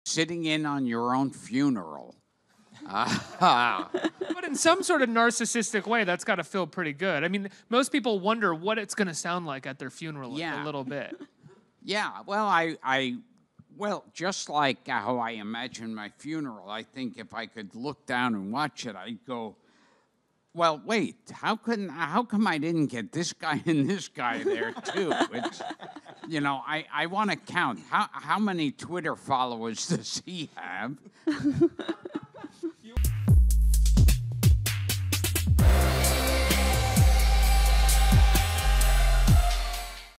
Gilbert Gottfried Says "Watching My Documentary Was Like Being Alive At My Funeral!"
Gilbert Gottfried talks about what he thought when he watched his documentary, "Gilbert," and explains why he didn't deserve a documentary...yet.